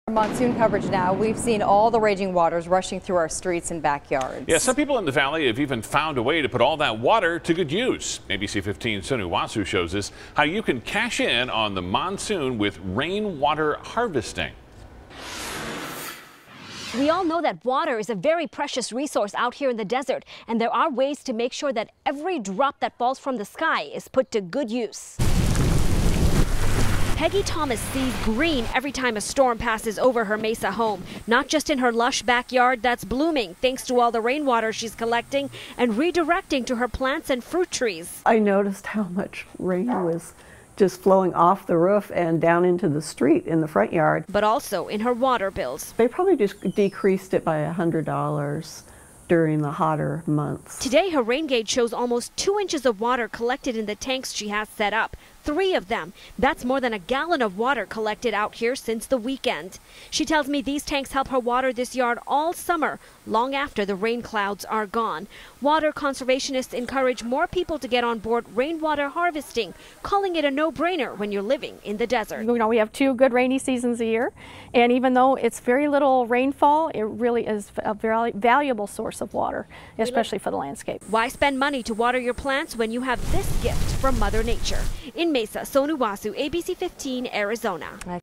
How you can cash in on the monsoon with rain water harvesting
We've seen all the raging waters rushing through our streets and backyards, and some people are cashing in on it!